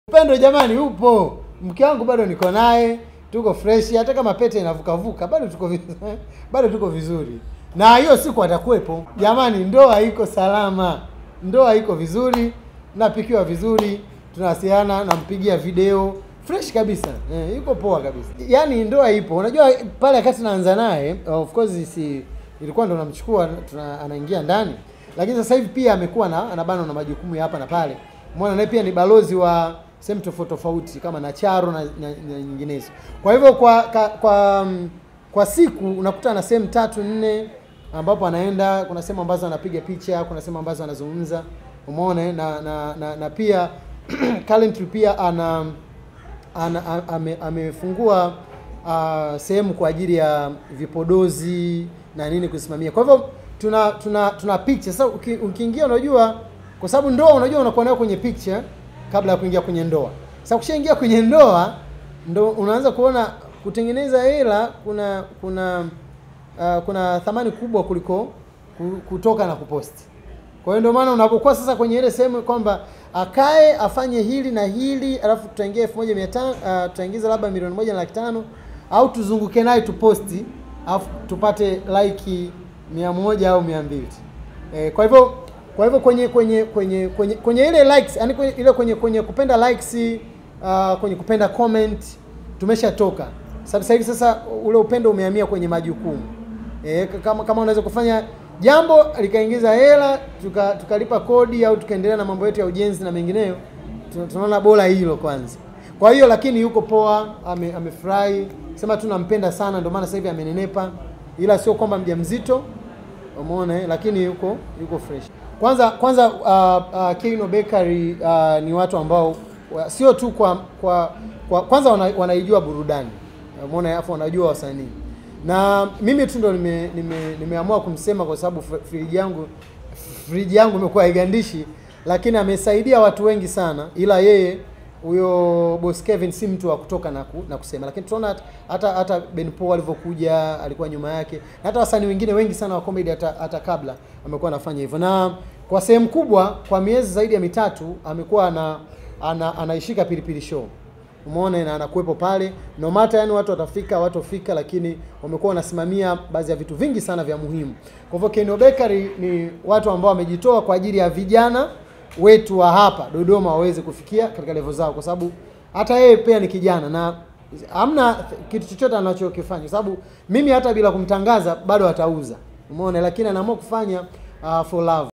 VIDEO:MC PILI PILI AELEZA SABABU ZA KUTO ONEKANA NA MKE WAKE
MKALI wa vichekesho vya jukwaani, Emmanuel Mathias 'MC Pilipili'amefunguka sababu za yeye kutoonekana na Mke wake pamoja kama ilivyokuwa huku nyuma ambapo walikuwa wanaambatana Pamoja kwenye Matukio tofauti.

Akizungumza na Dodoma Tv kuelekea tukio lake Christmas Comedy Festival,Mkali huyo amesema watu wategemee kumuona mke wake siku hiyo lakini amefunguka zaidi kinachokwamisha kutoambatana na mke wake kwenye Matukio mengi kwa sasa.